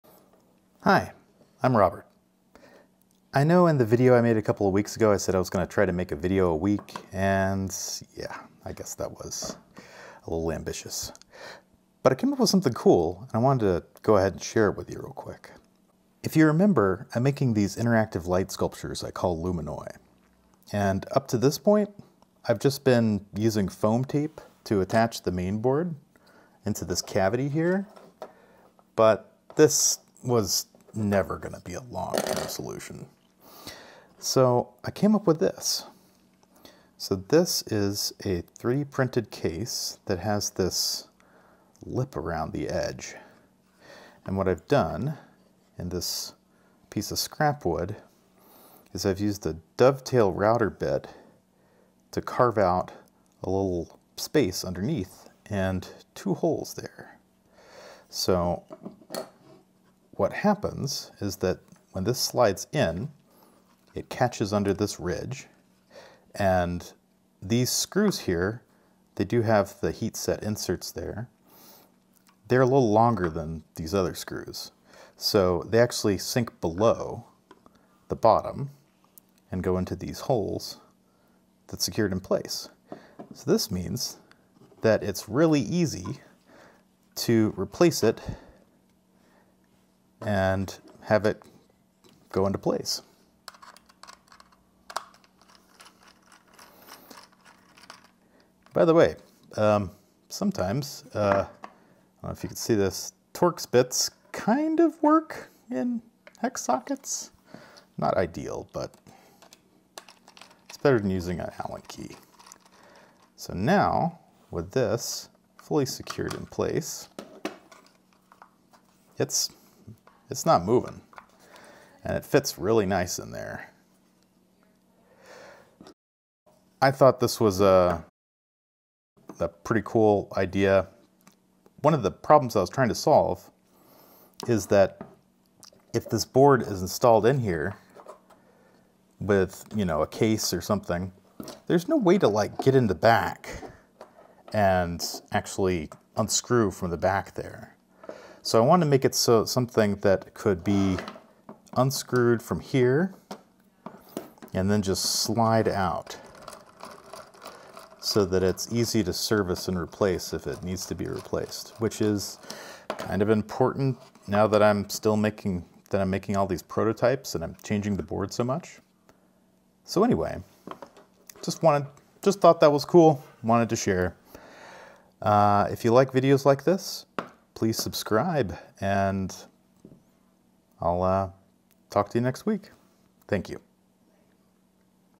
Designing for Repairability: Mounting the main board
In this video, I show a clever mounting solution for the mainboard on my Lumanoi interactive light sculptures that makes it easy to remove/replace and is very secure.